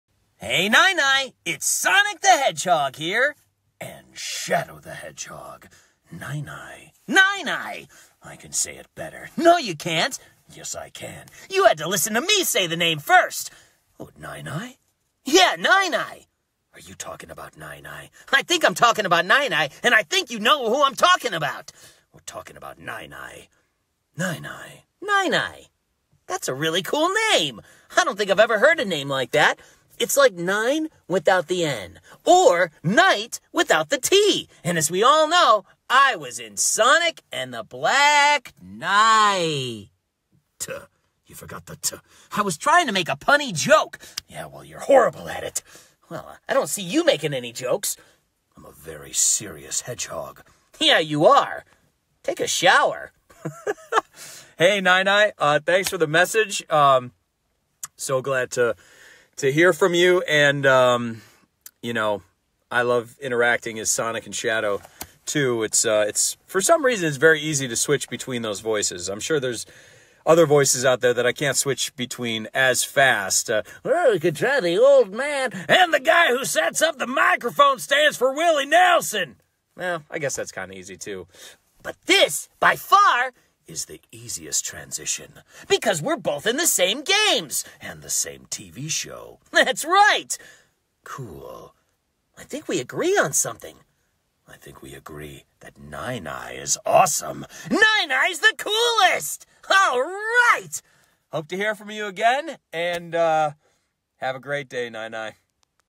Jason Griffith Cameo (Sonic and Shadow shouting my name and bickering)
It was fun talking to Jason Griffith a bit.
We only started texting because he didn't know how to pronounce my name haha.

Very silly and funny too.
Was worth getting him to do this.